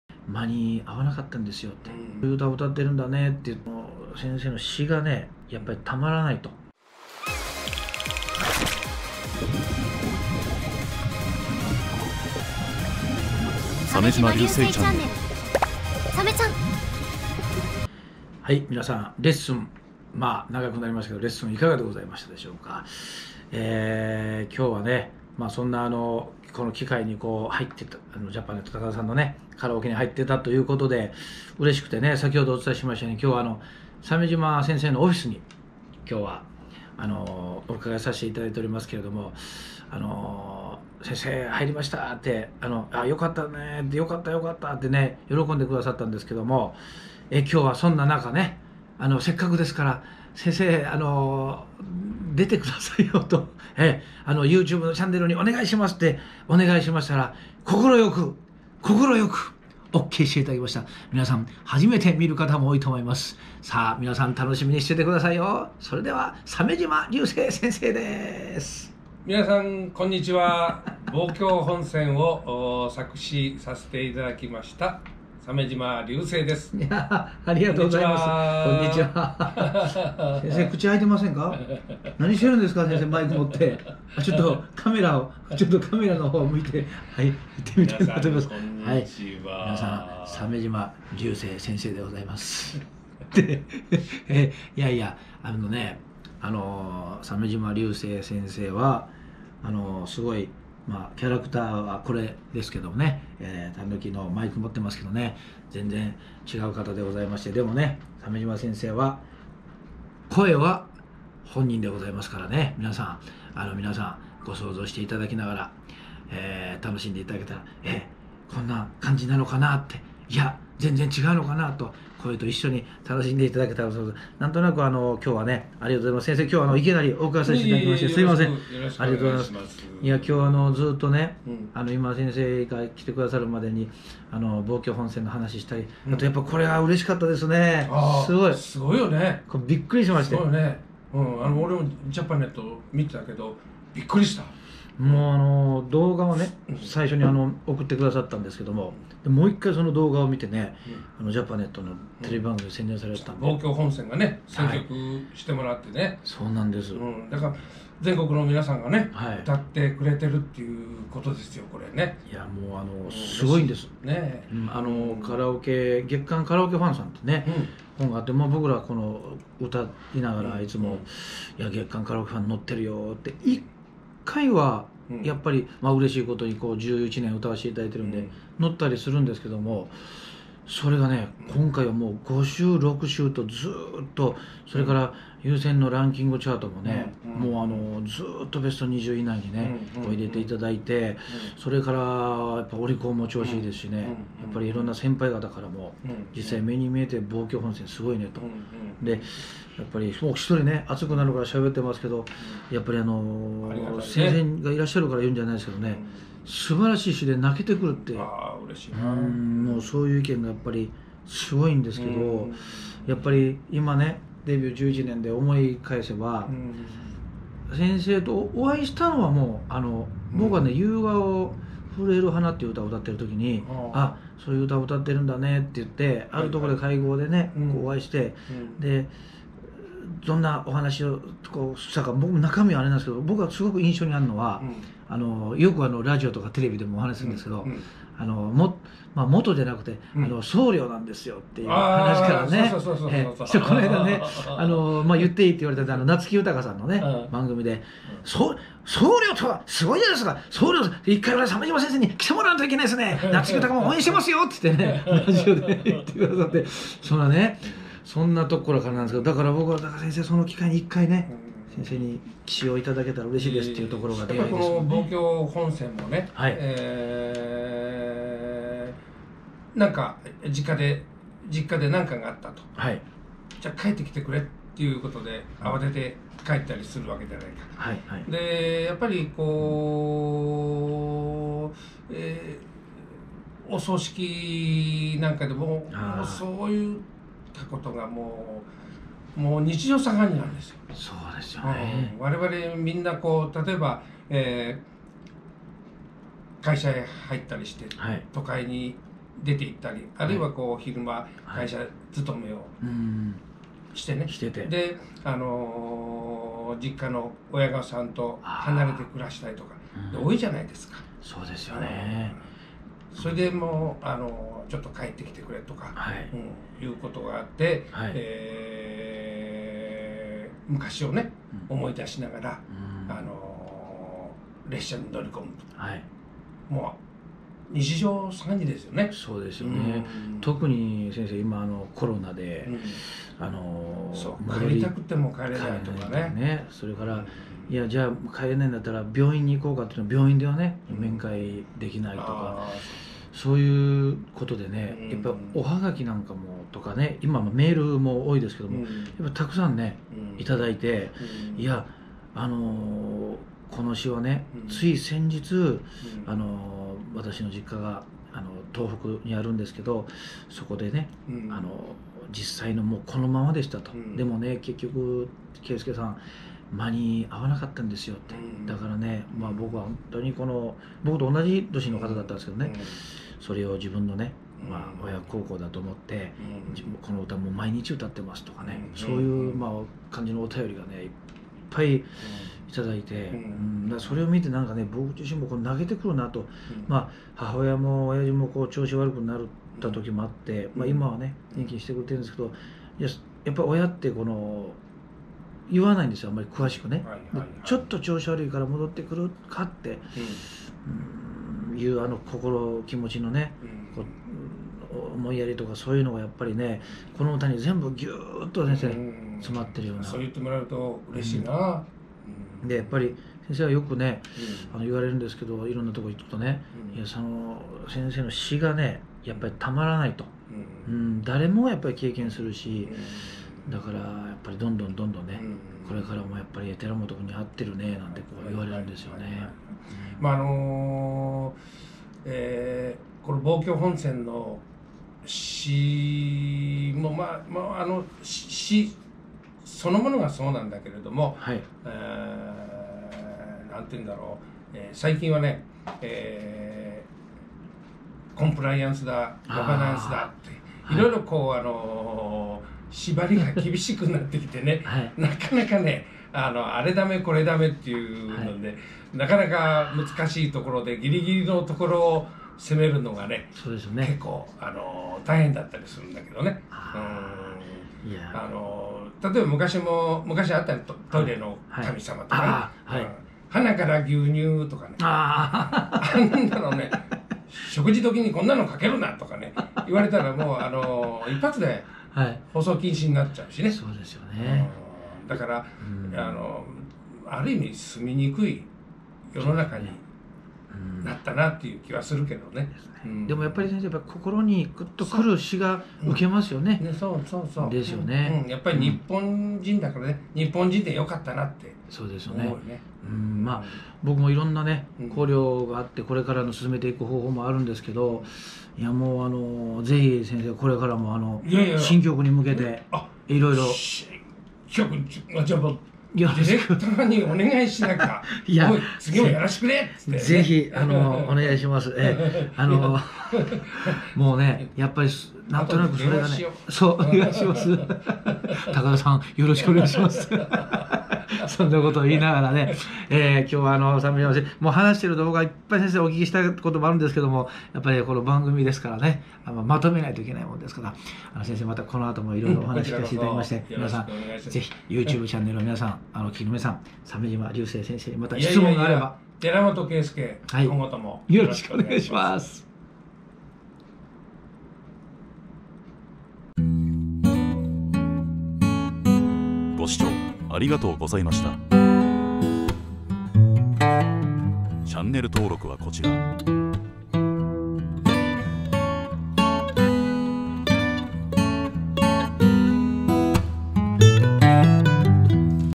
寺本圭佑、鮫嶋琉星対談！
レッスンに引き続き第二弾。
鮫嶋琉星と寺本圭佑との対談編。